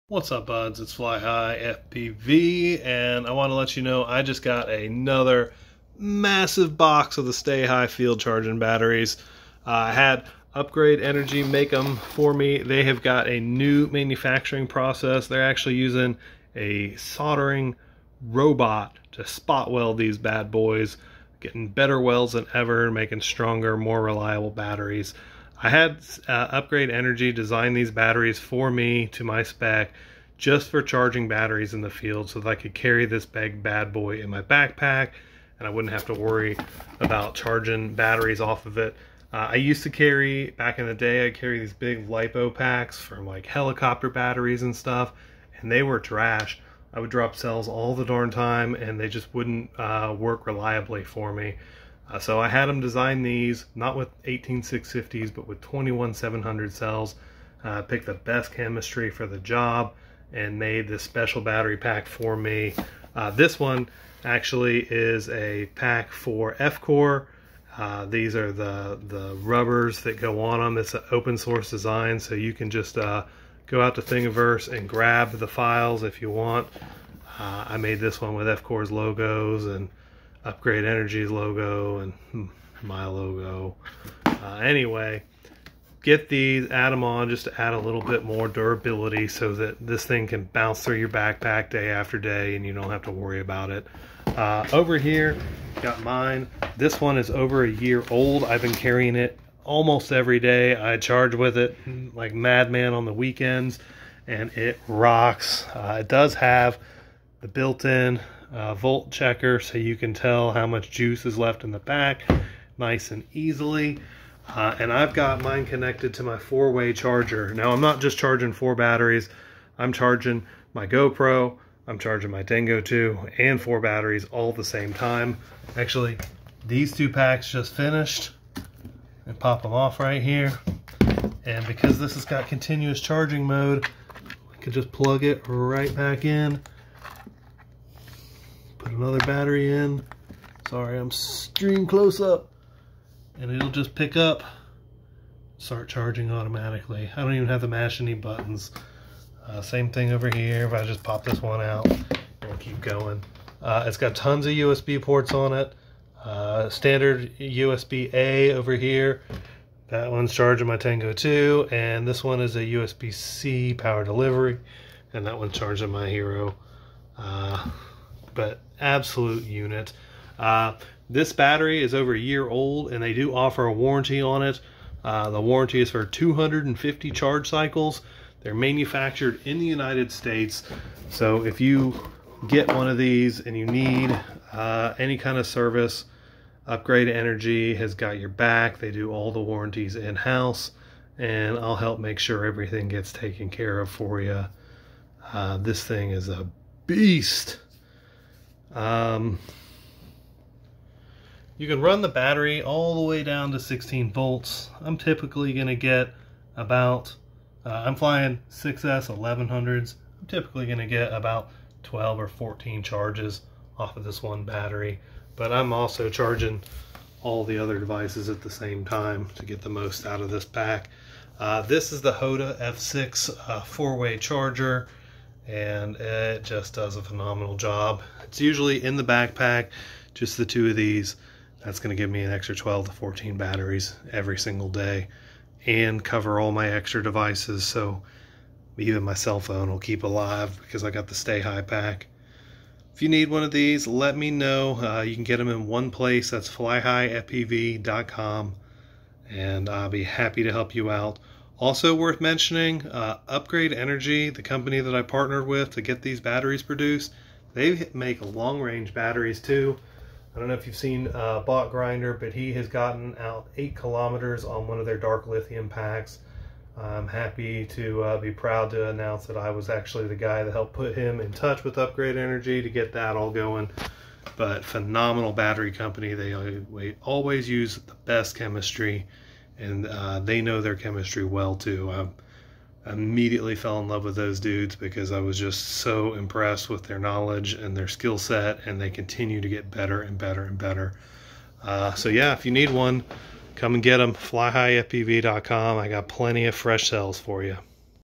Charge all the Lipos with the Stay High Field Charging Pack from Fly High FPV and UpgradeEnergy!
I made the ultimate field charging battery for FPV Drones! 6S and 15,000mAh, 378Wh!